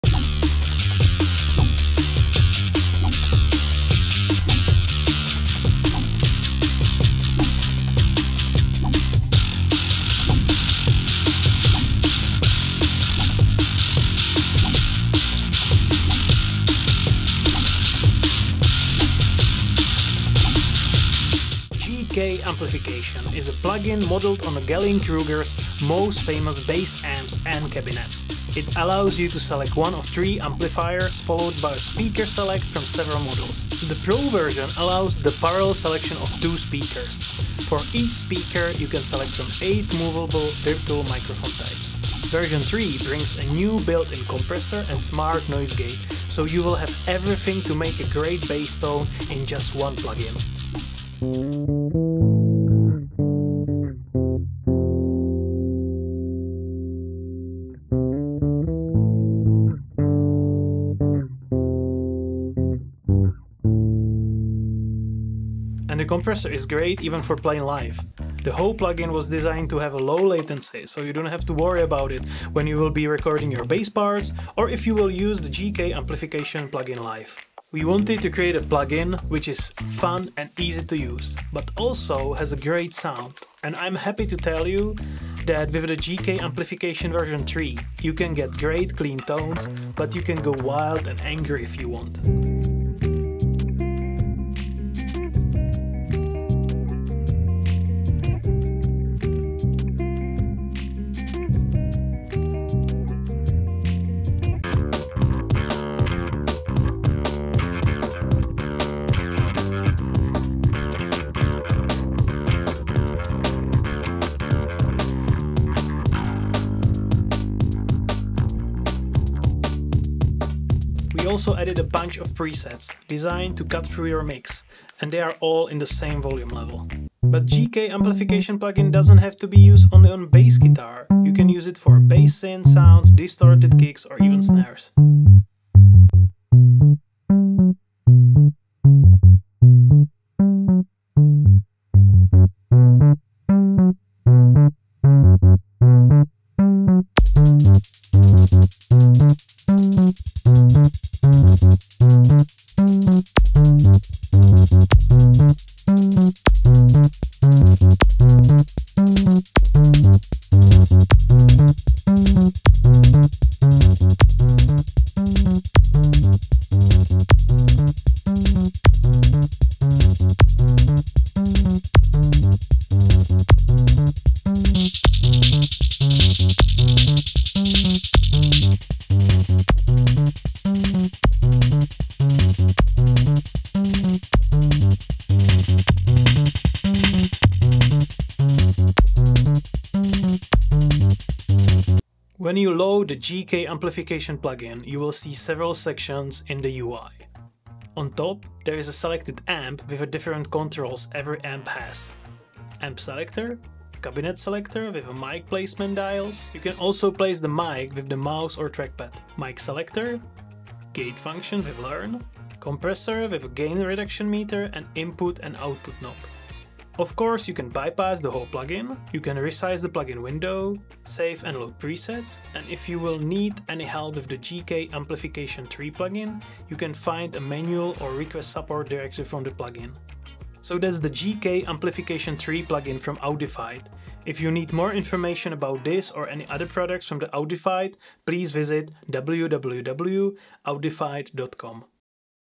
GK Amplification 3 Pro - plugin walkthrough | Audified
Explanation of the functions followed by audio demonstrations and the possibilities of using the plugin.

Video by Patrik Veltruský